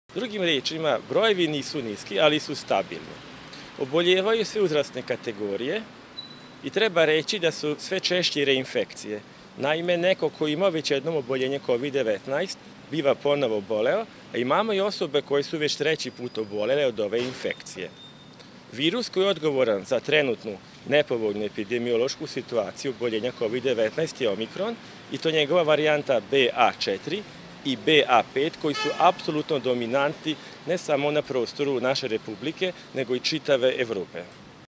Epidemiološka situacija i dalje nepovoljna, ali stagnira
Epidemiološka situacija na teritoriji Subotice u vezi sa virusom kovid 19 ocenjuje se kao nepovoljna, poručuju iz Zavoda za javno zdravlje Subotica i dodaju da je ipak ohrabrujuća činjenica da dnevni broj obolelih već nekoliko nedelja stagnira i iznosi oko 100 zaraženih.